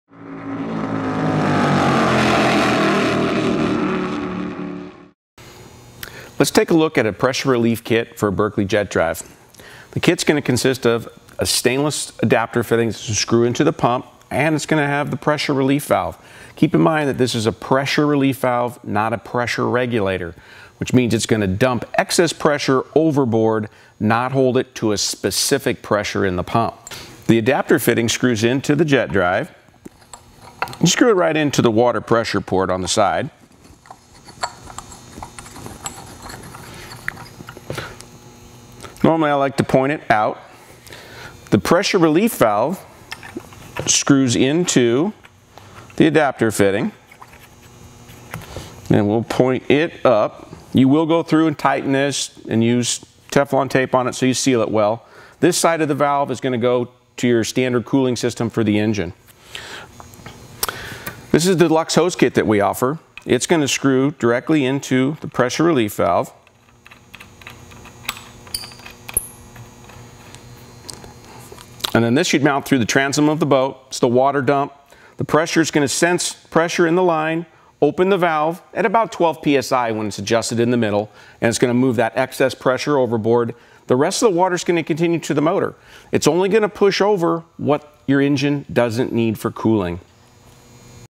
Adding a Pressure Relief Valve Kit to a Berkeley Jet Drive
Rick shows us how to install a helpful Berkeley water pressure relief valve kit and other accessories to a Berkeley Jet Pump